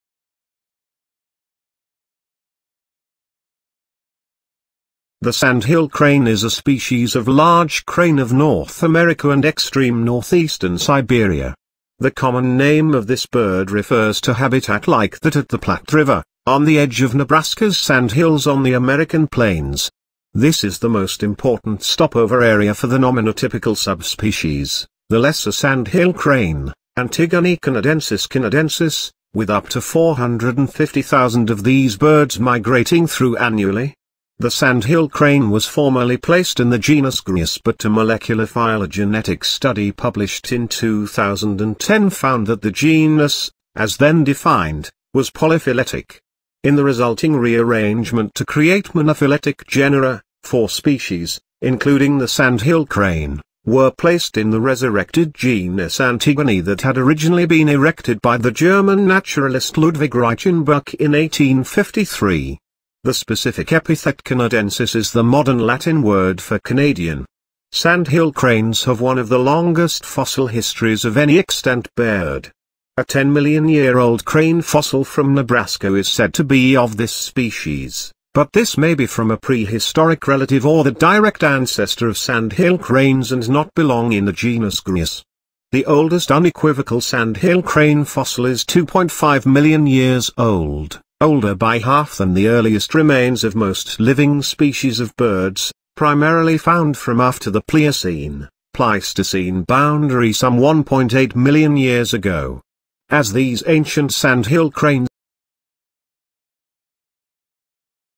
Interesting facts about crane sandhill by weird square
The sandhill crane  is a species of large crane of North America and extreme northeastern Siberia. The common name of this bird refers to habitat like that at the Platte River, on the edge of Nebraska's Sandhills on the American Plains. This is the most important stopover area for the nominotypical subspecies, the lesser sandhill crane (Antigone canadensis canadensis), with up to 450,000 of these birds migrating through annually.
The sandhill crane was formerly placed in the genus Grus but a molecular phylogenetic study published in 2010 found that the genus, as then defined, was polyphyletic. In the resulting rearrangement to create monophyletic genera, four species, including the sandhill crane, were placed in the resurrected genus Antigone that had originally been erected by the German naturalist Ludwig Reichenbach in 1853. The specific epithet canadensis is the Modern Latin word for "Canadian".
Sandhill cranes have one of the longest fossil histories of any extant bird. A 10-million-year-old crane fossil from Nebraska is said to be of this species,  but this may be from a prehistoric relative or the direct ancestor of sandhill cranes and not belong in the genus Grus. The oldest unequivocal sandhill crane fossil is 2.5 million years old, older by half than the earliest remains of most living species of birds, primarily found from after the Pliocene/Pleistocene boundary some 1.8 million years ago. As these ancient sandhill cranes varied as much in size as present-day birds, those Pliocene fossils are sometimes described as new species. Grus haydeni may have been a prehistoric relative, or it may comprise material of a sandhill crane and its ancestor.
Sandhill cranes are fairly social birds that usually live in pairs or family groups through the year. During migration and winter, unrelated cranes come together to form "survival groups" that forage and roost together. Such groups often congregate at migration and winter sites, sometimes in the thousands.
Sandhill cranes are mainly herbivorous, but eat various types of food, depending on availability. They often feed with their bills down to the ground as they root around for seeds and other foods, in shallow wetlands with vegetation or various upland habitats. Cranes readily eat cultivated foods such as corn, wheat, cottonseed, and sorghum. Waste corn is useful to cranes preparing for migration, providing them with nutrients for the long journey. Among northern races of sandhill cranes, the diet is most varied, especially among breeding birds. They variously feed on berries, small mammals, insects, snails, reptiles, and amphibians. Animal planet | Discovery | Netflix | Documentary | Animal World